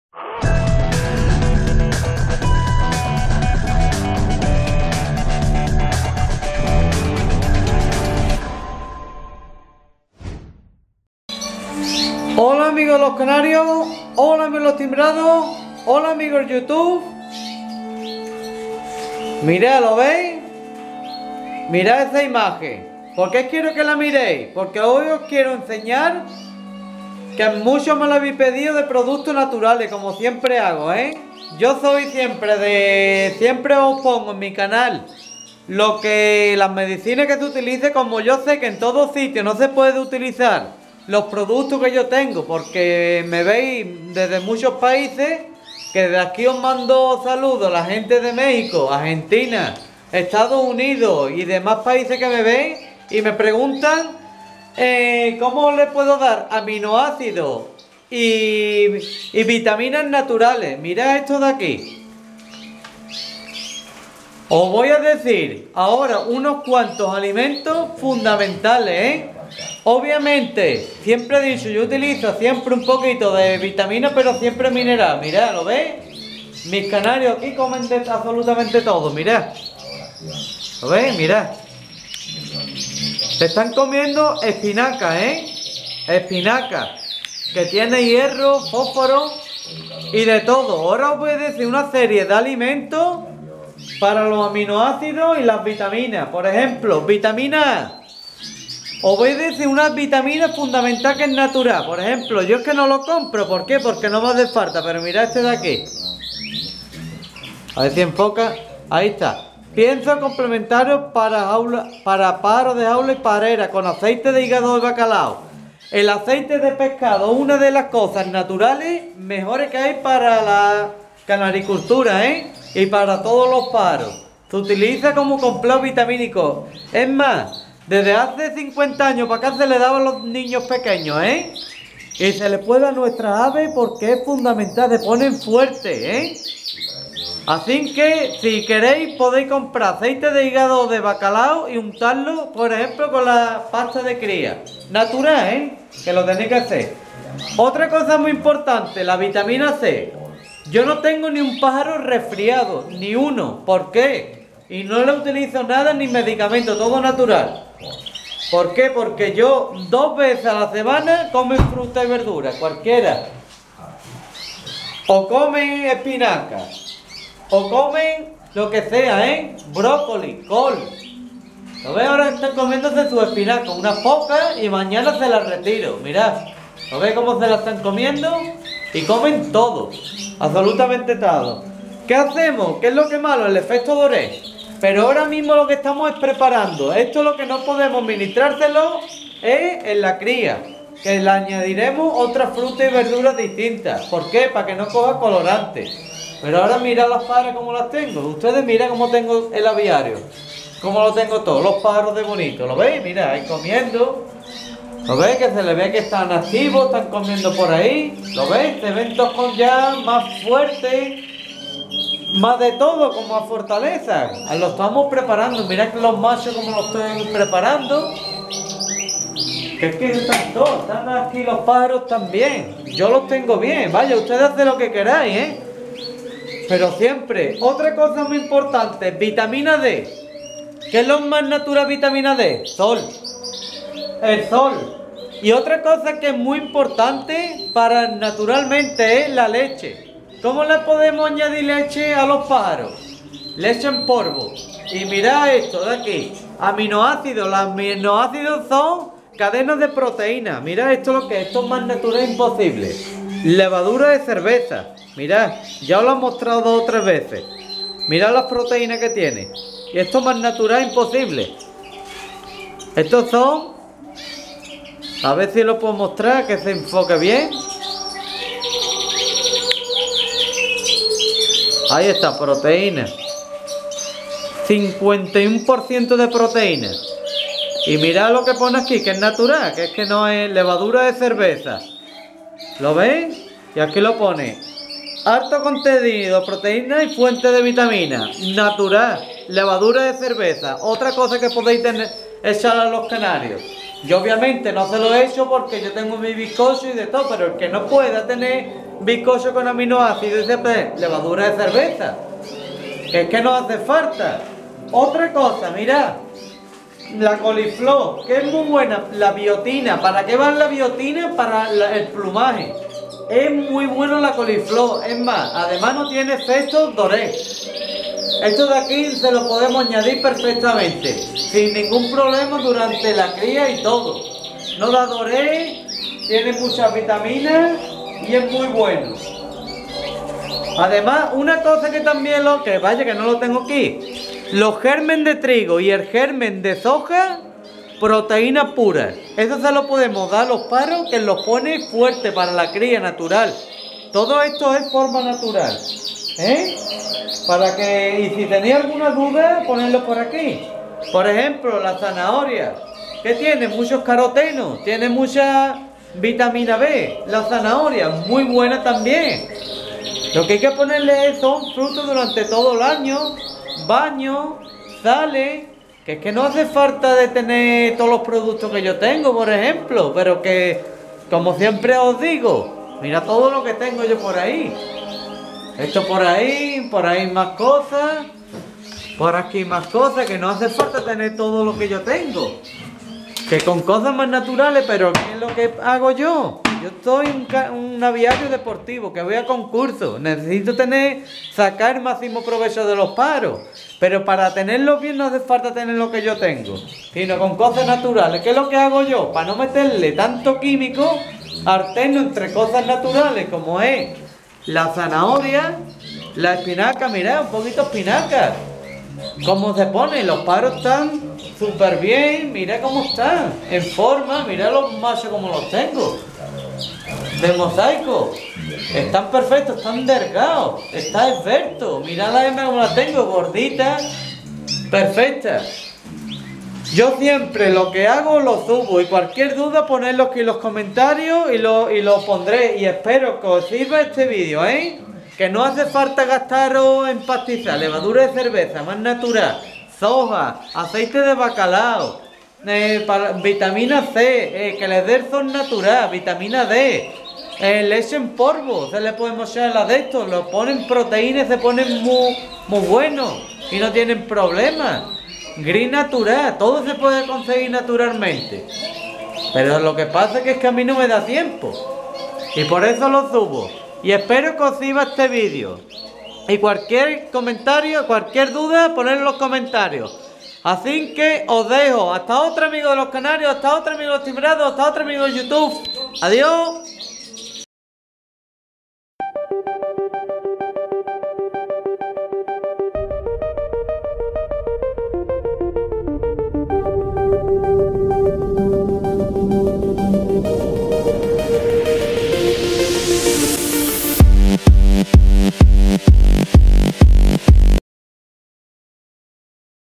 VITAMINAS Y AMINOACIDOS NATURALES (HD)
ADEMAS DE CANARIOS NOS VALE PARA NOSOTROS MISMO ENTRE MAS COSAS NATURALES CONSUMAMOS DE FRUTA  Y VERDURA MEJOR ESTAREMOS NO ES LO MISMO UNA PASTILLA DE HIERRO QUE COMER ESPINACAS POR PONER UN EJEMPLO ASI QUE ESPERO QUE OS GUSTE, SALUDOS XD